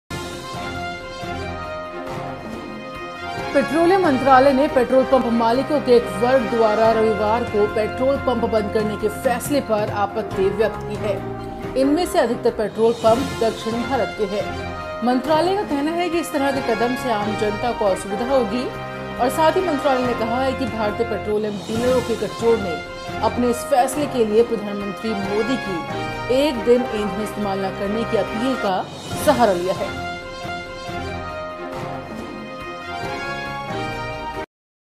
संडे को पेट्रोल पंप बंद पर सरकार सख़्त
The oil ministry has denounced the decision of a section of petrol pump operators, mostly in South India, to keep outlets shut on Sundays, saying such a move will inconvenience the general public.